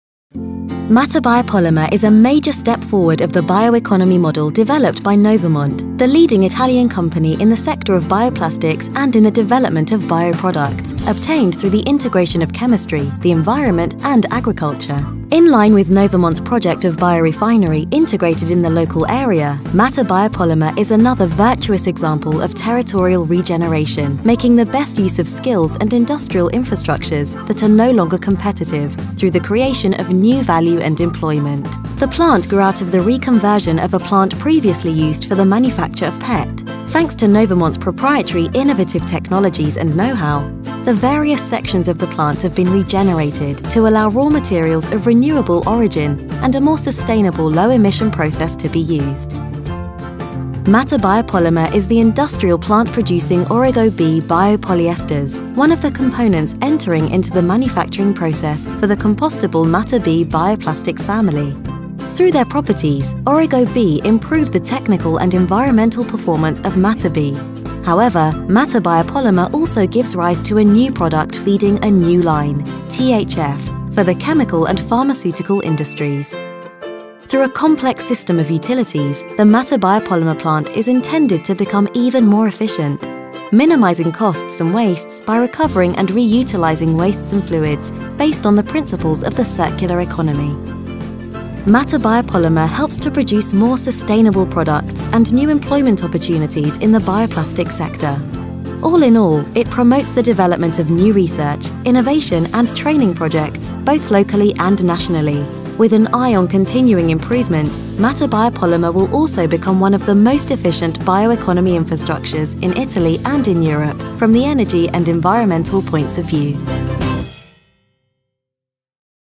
Mater-Biopolymer: the regeneration comes one
Mater-Biopolymer is the company controlling the industrial site in Lazio, Patrica (FR). Novamont has acquired the 100% of the business in 2017. Mater-Biopolymer produces Origo-Bi biopolyesters through a line completely modified over the years using a proprietary Novamont technology. The company is also involved in the development of new biopolymers.